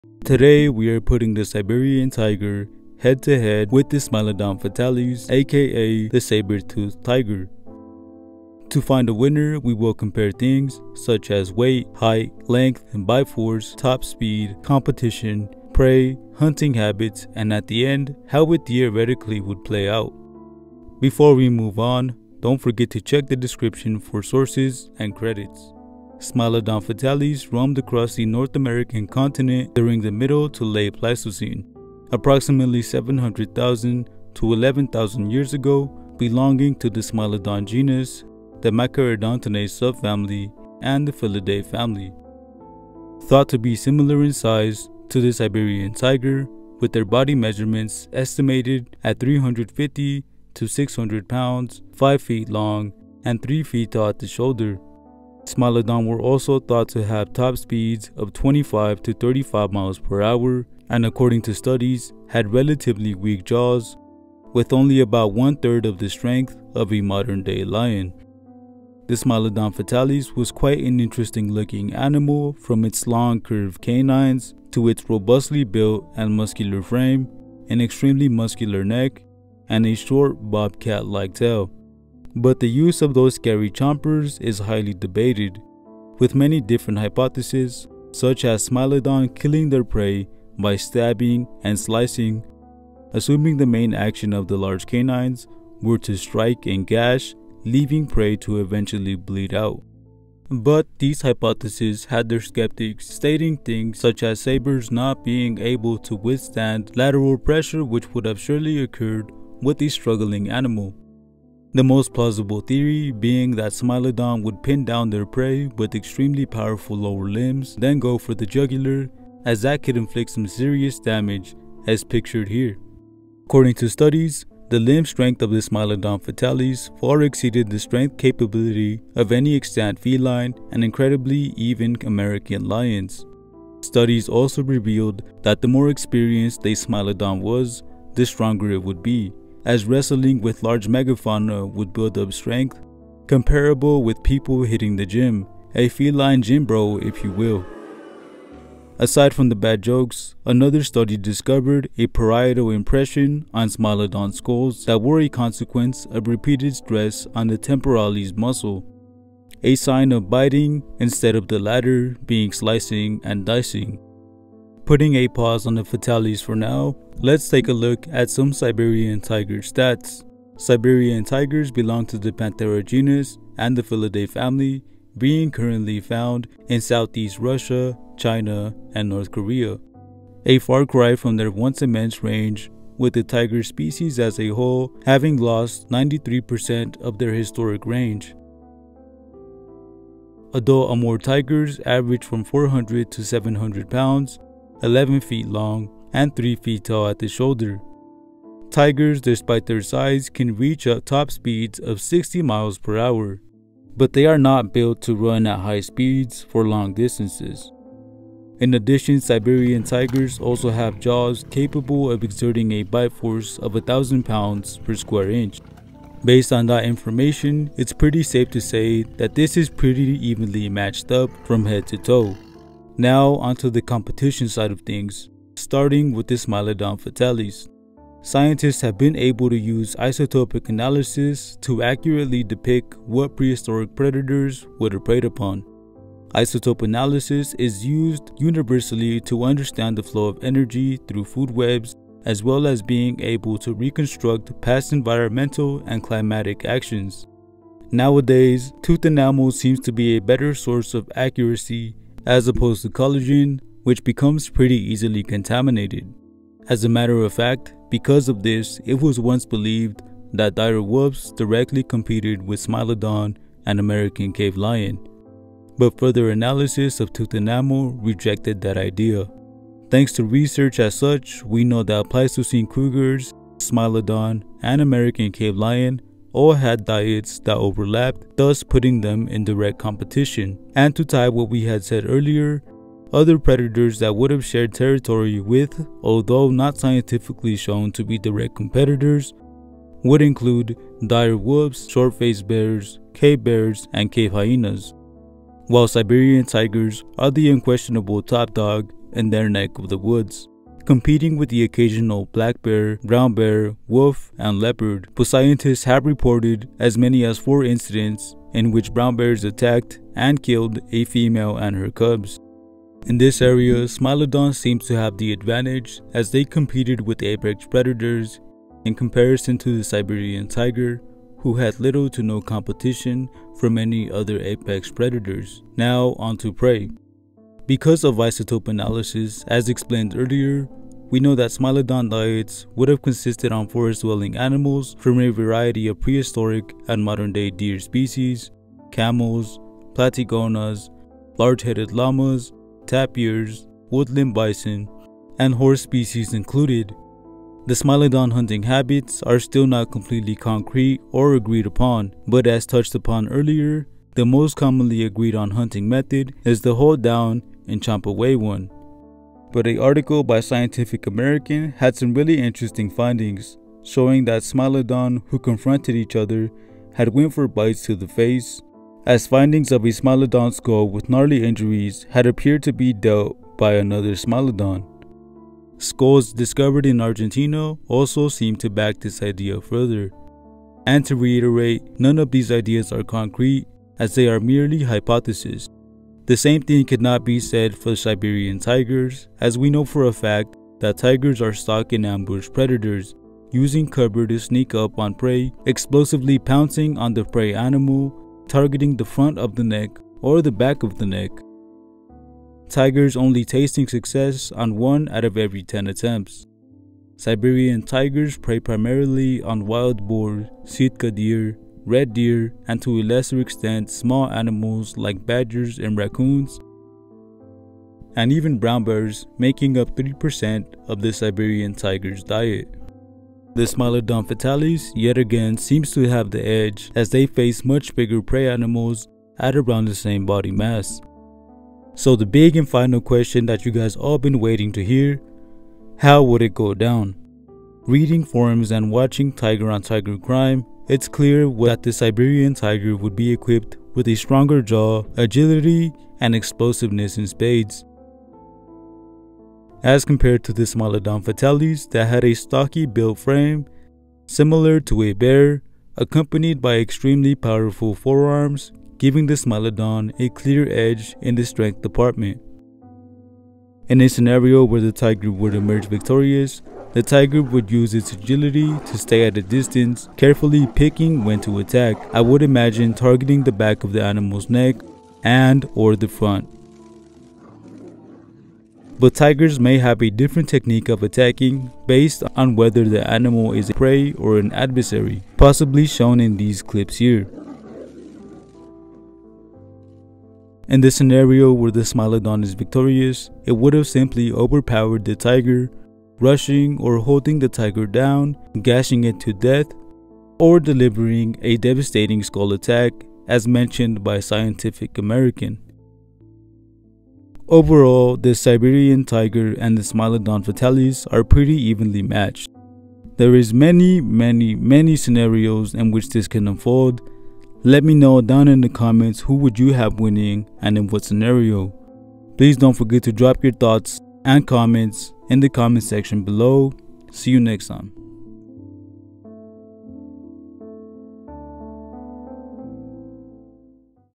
Siberian Tiger vs The Saber-toothed Tiger
The
Siberian tiger vs the saber tooth tiger or saber tooth cat.please be aware that these names are generic as smilodon are not closely related to tigers and other types of saber-toothed cats have existed .a match up comparing things such as weight ,length,competition,bite force,top speed and Ex.keep in mind that the smilodon fatalis is one of 3 species of the smilodon genus.

Please note We do not own some of the pictures and videos show. In this informational video if you are the owner and would like your name to be linked or something of the sort get in contact with the us.

Full video running tigers

Fighting tigers

Chomping tiger

Sources on tiger

Smilodon fatalis sources